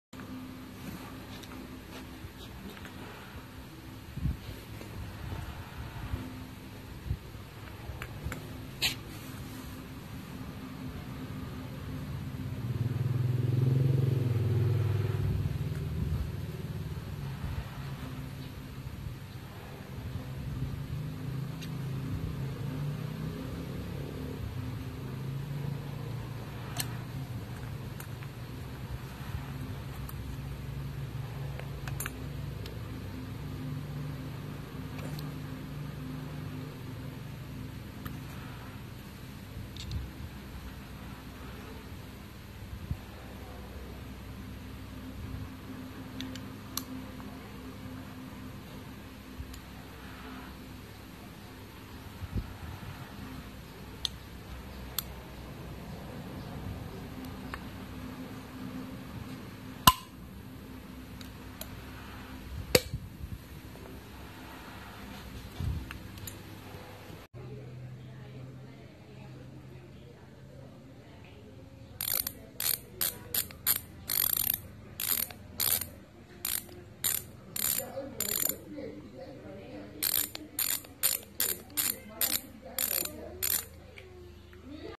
XBP-HB01M Metalmax Wheel Hubs 32H
Total Weight included QR; 544gram
Durable
Lightweight
Can change to Thru-Axle F/R
32 Holes
Material; Alloy
4 Sealed Bearing